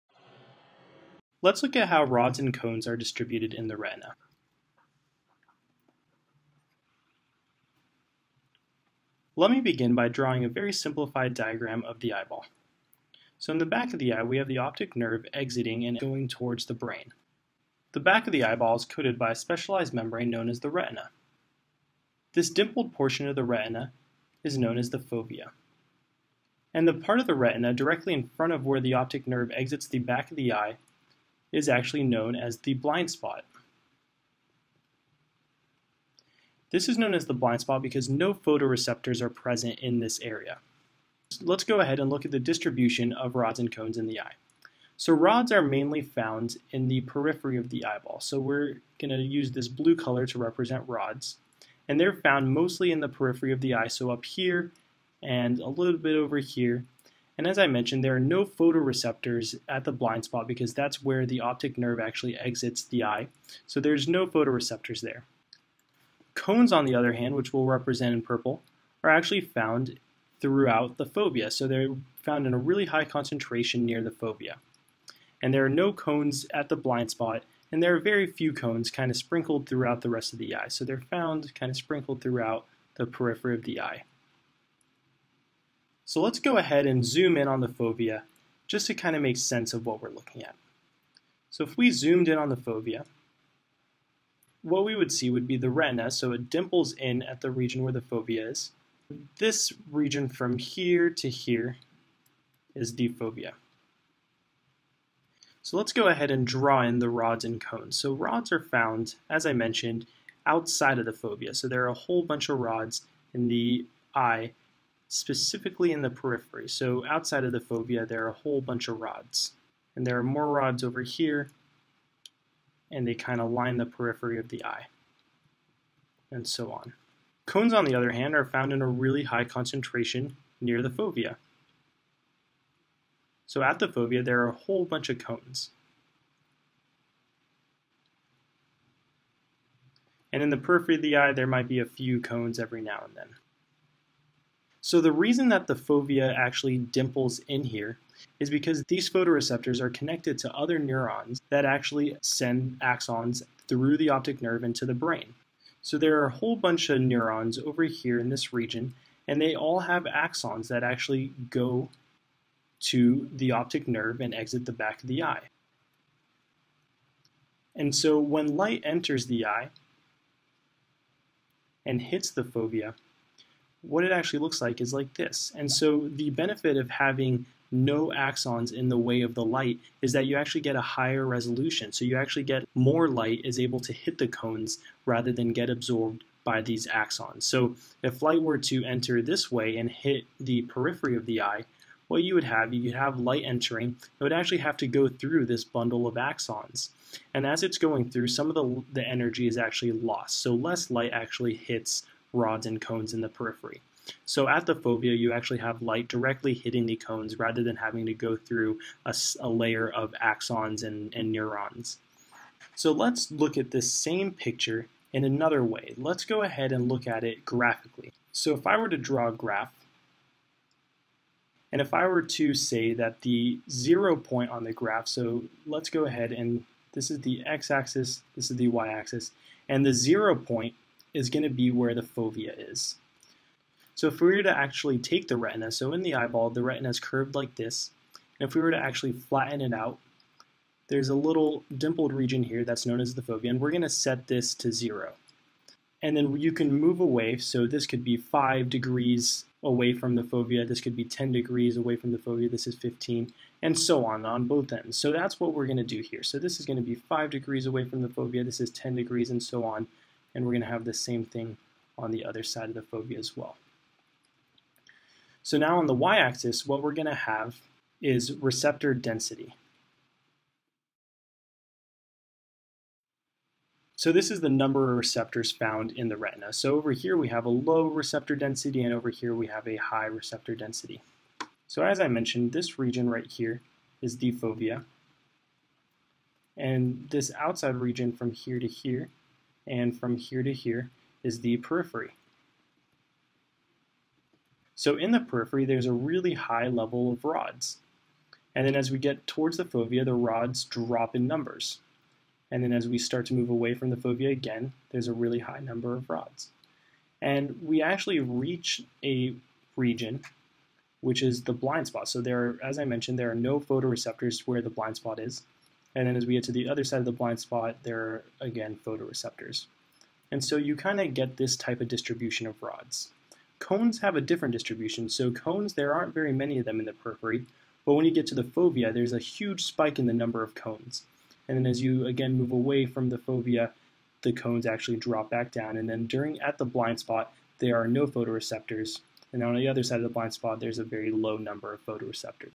Photoreceptor distribution in the fovea | Processing the Environment | MCAT | Khan Academy
In this video, I explore how photoreceptors (rods and cones) are distributed in the fovea. By Ronald Sahyouni. Created by Ronald Sahyouni.